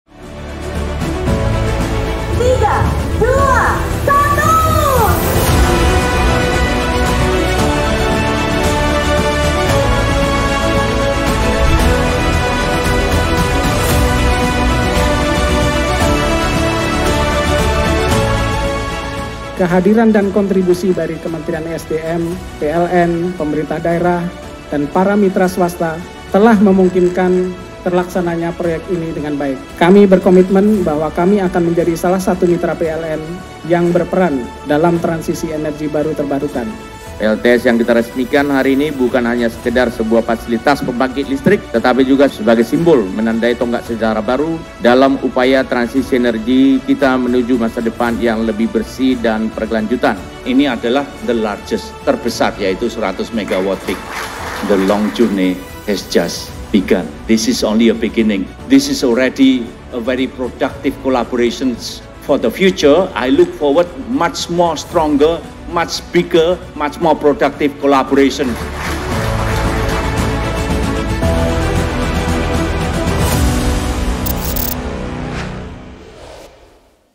Kolaborasi PLN & Aruna PV Bangun PLTS Ground-Mounted Terbesar di Indonesia
Electrizen, PLN melalui anak usahanya, PLN Batam bersama PT Aruna Cahaya Pratama (Aruna PV) sukses mengoperasikan Pembangkit Listrik Tenaga Surya (PLTS) ground-mounted terbesar di Indonesia dengan kapasitas 100 Megawatt peak (MWp) di Kawasan Industri Kota Bukit Indah, Kabupaten Purwakarta, Jawa Barat, Rabu (28/08).

Berdiri di atas lahan dengan total luas 80 hektar, PLTS yang dioperasikan PT Aruna Hijau Power (AHP), joint venture antara PLN Batam dengan Aruna PV ini diperkirakan mampu menghasilkan 150 GWh energi bersih pertahun atau setara dengan pengurangan emisi sebesar 118.725 ton CO2.

Pengoperasian pembangkit ramah lingkungan ini merupakan wujud kolaborasi PLN bersama pihak swasta dalam negeri dalam upaya penyediaan energi bersih dan pengurangan emisi karbon sekaligus meningkatkan daya saing industri dalam negeri melalui pemanfaatan listrik hijau.